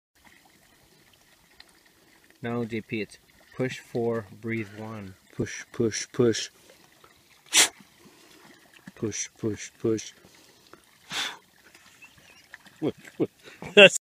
CPR on a Frog
Those of you that know me, know that I am somewhat of a humanitarian and all around good person ... so naturally when I came upon this Pseudacris triseriata/feriarum who had apparently drowned and most likely alcohol was involved ...

... I was compelled to stop and render aid.

Although I did try and resuscitate the creature it was a hopeless endeavor and little frog went on to wherever good frogs go ...

... unless he was a bad frog ...

... then I guess he would have went to "Frog Hell" .

... which I imagine would be a shallow pond full of "Great Blue Herons" ... and snakes ... lots of snakes

"Never Give Up" frog ... just like the t-shirt says .